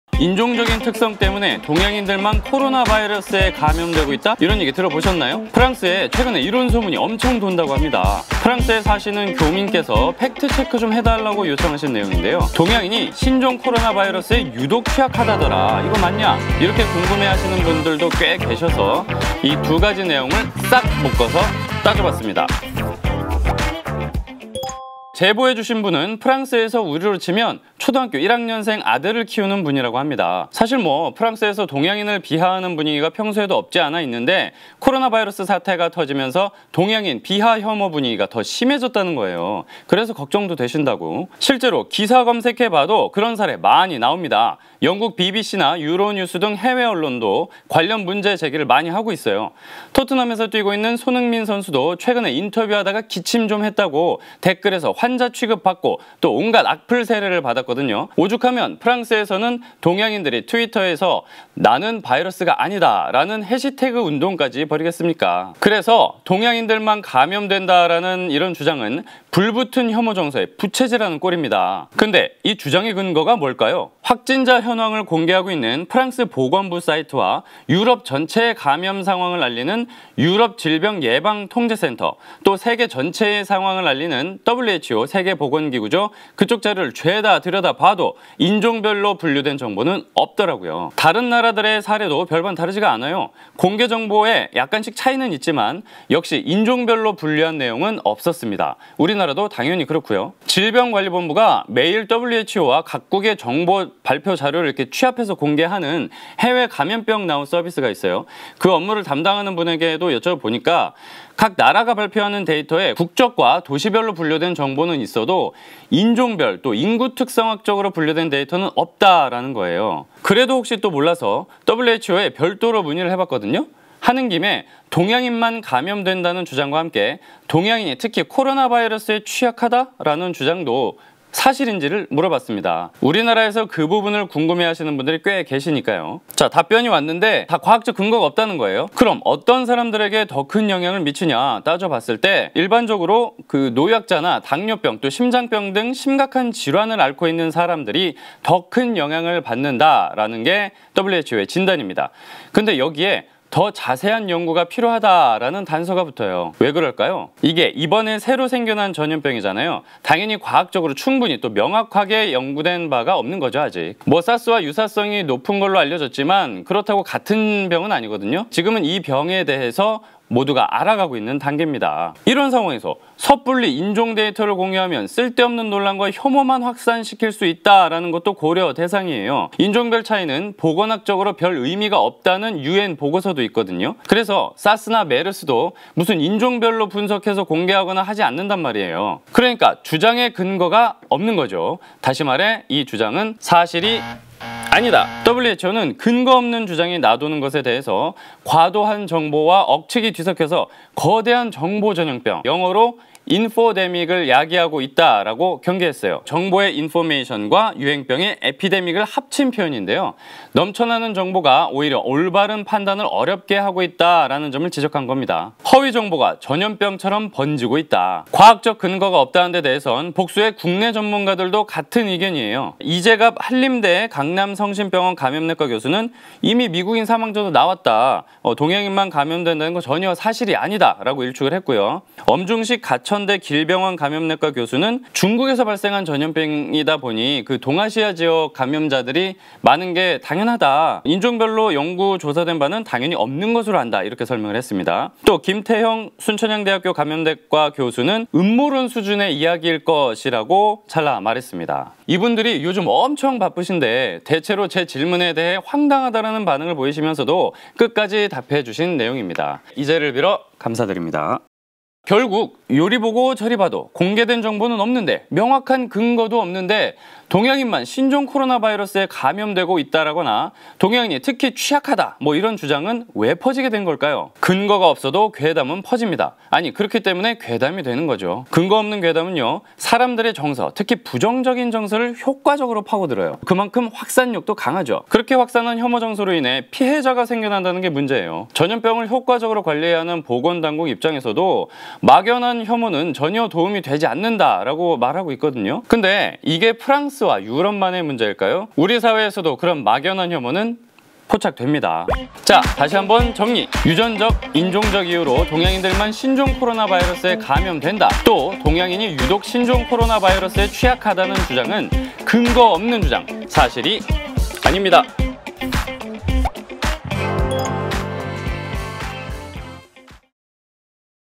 [팩톡] 인종적 특성으로 동양인이 특히 코로나바이러스에 취약하다??
프랑스 교민이 사실확인을 요청한 사안입니다.
프랑스에서 최근 "유전적·인종적·체질적 이유로 동양인들만 코로나바이러스에 감염되고 있다"는 소문이 돌기 시작했대요~!

안 그래도 동양인에 대한 비하 정서가 조금 있는데, 신종 코로나바이러스 사태로 비하는 물론 혐오 정서까지 강화되고 있어 걱정이라는 겁니다~ ㅠ.ㅠ  동양인에 대한 혐오와 차별 사례는 국내외 기사들을 통해 익히 알려졌는데요...

결국, 저런 주장이..불붙은 동양인 혐오 정서에 부채질을 하는 것 아닌가요??;;;  그래서 꼭 따져봐야 할 것 같은데요!

 "신종 코로나바이러스는 동양인만 걸린다"라거나 "동양인이 유독 취약하다"는 주장은 사실일까요?  팩톡이 다각도로 따져서 5분 순삭 영상으로 설명해드립니다~!